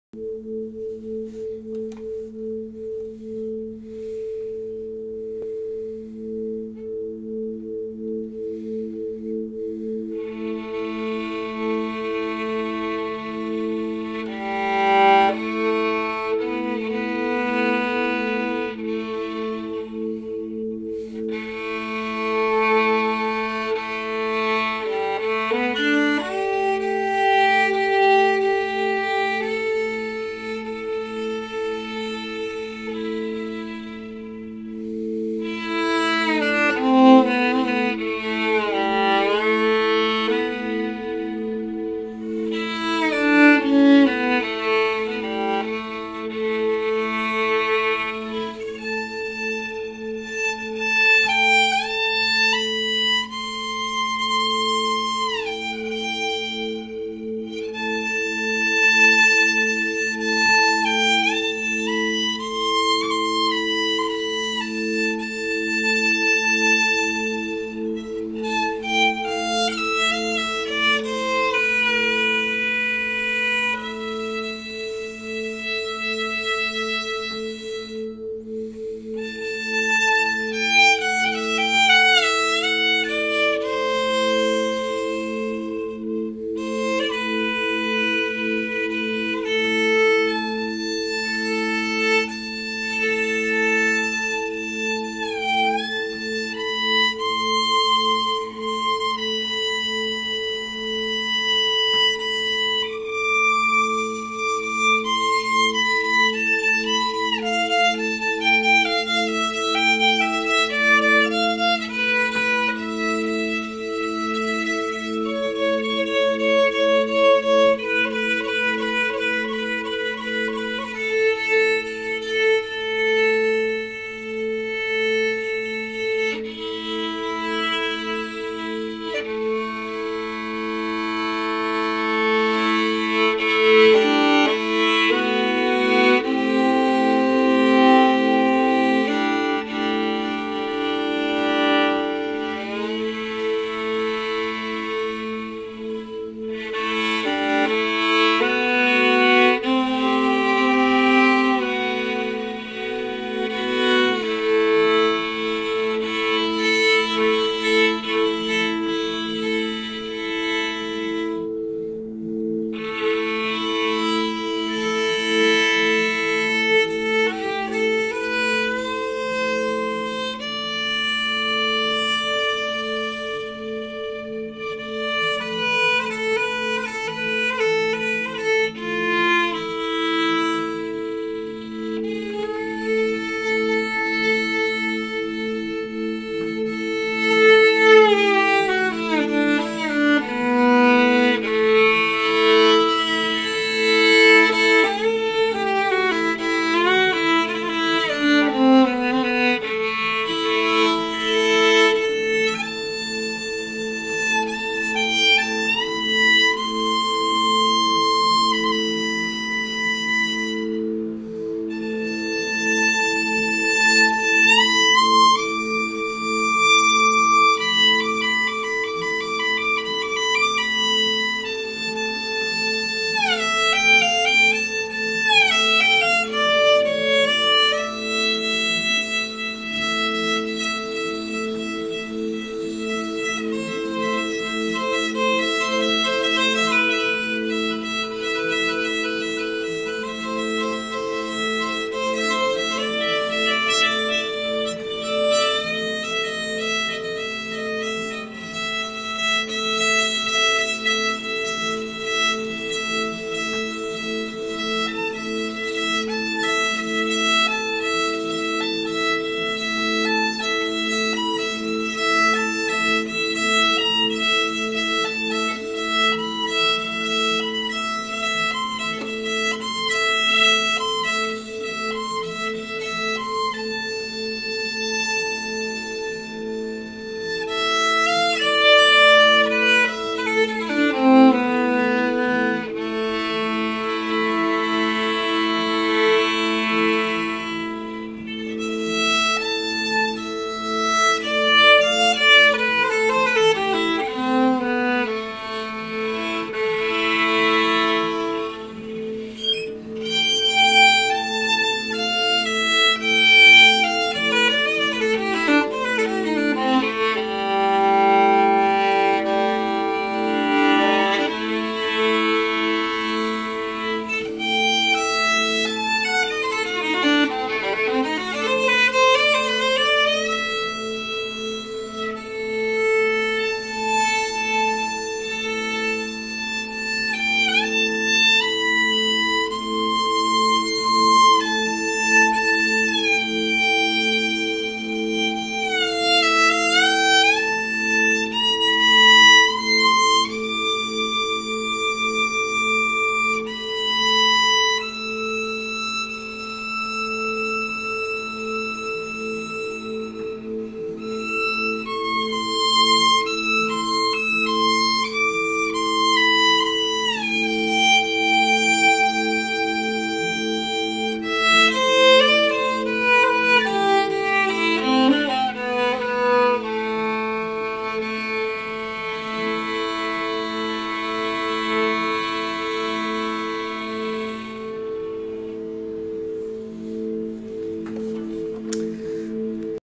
Improvised String Meditation 5/2/2020
Moving into 6 weeks of social distancing here in NYC.  Soothing and healing through these meditations.